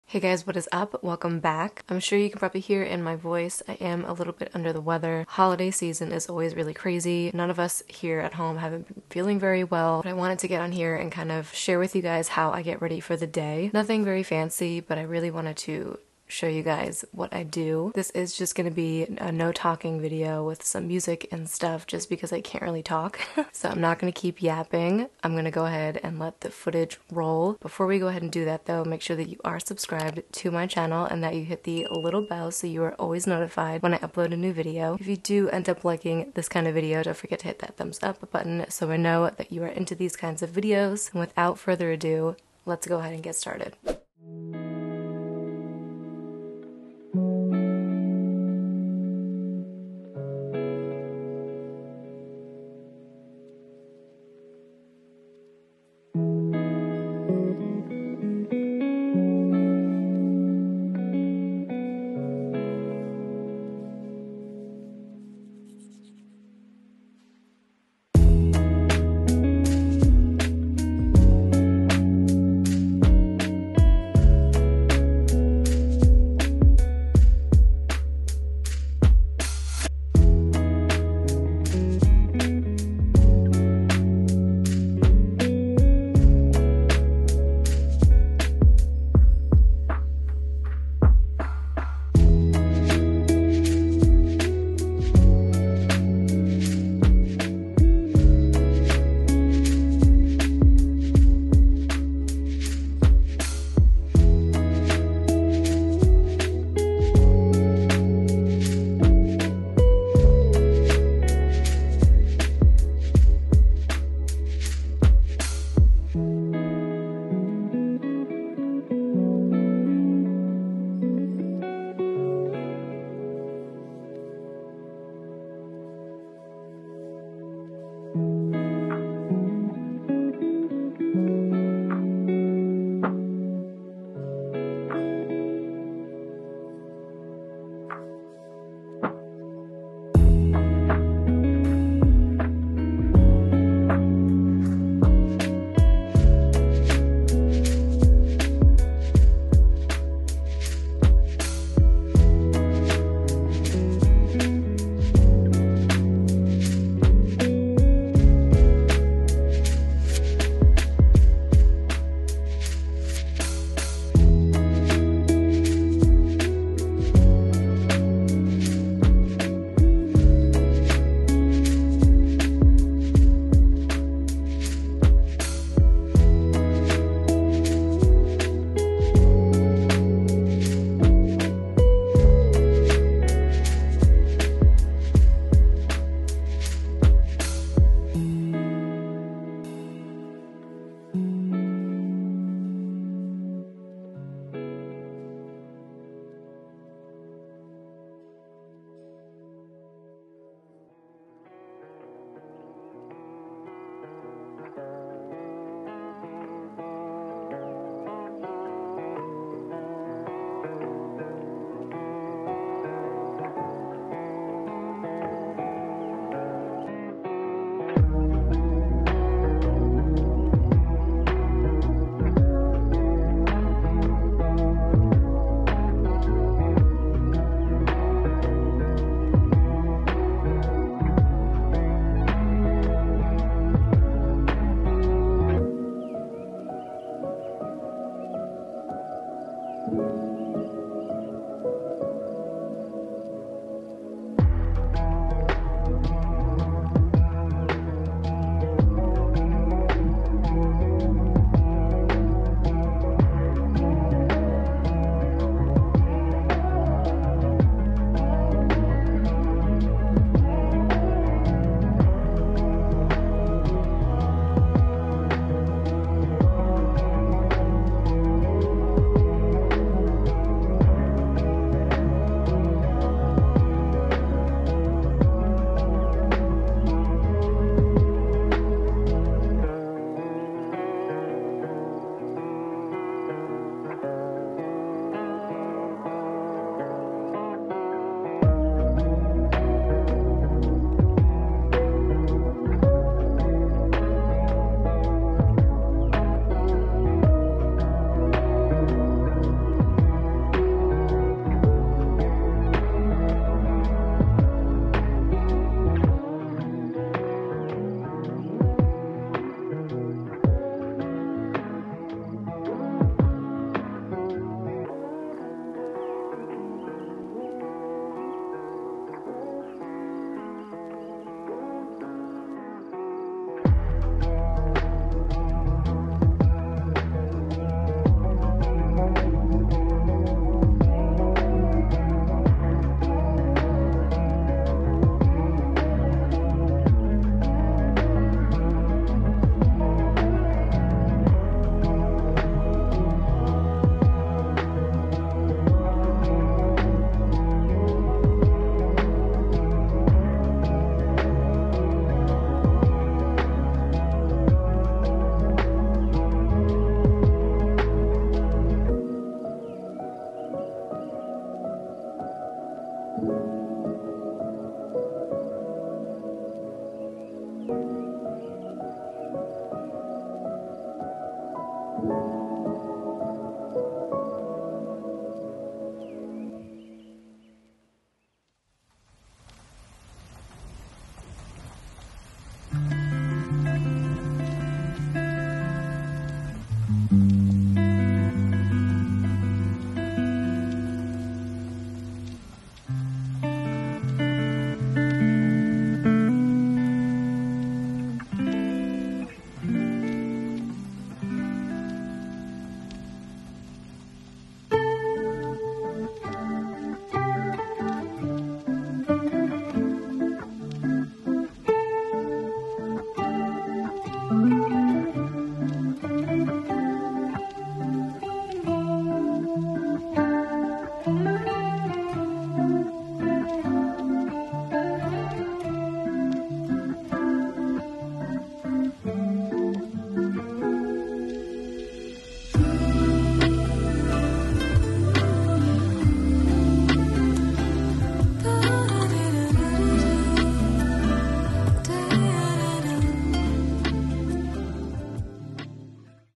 let's get ready for the day together | FALL EDITION 🍂 | caitlinxtine
let's get ready for the day together | FALL EDITION 🍂

hellllloooo beautiful ppl! today i will be showing you my daily basic makeup routine so let's get ready for the day together FALL EDITION! this tutorial will feature my favorite no make makeup look. i will be showing you guys how to get the perfect daily soft glam for beginners and what the best products to use are for daily makeup. i will also be showing you what the best foundation is for my oily skin for everyday. this video is full of my daily makeup favorites and products that i know are good and perform well. this video is very beginner friendly and will show you an in depth tutorial for how to get the perfect daily basic glam. i can't wait for you all to check out this video and for you to try it out for yourselves! love you all hope you enjoy xoxoxoxoxoxoxox

➭ 00:00 Intro
➭ 00:44 Skin Prep
➭ 02:14 Primer
➭ 02:26 Foundation
➭ 02:48 Cream Bronzer
➭ 03:01 Cream Blush
➭ 03:10 Concealer
➭ 03:56 Setting Powder
➭ 04:29 Bronzer
➭ 04:45 Blush
➭ 04:58 Eyes
➭ 05:24 Brows
➭ 05:32 Lips
➭ 06:20 Hair
➭ 06:48 Final Step & Final Look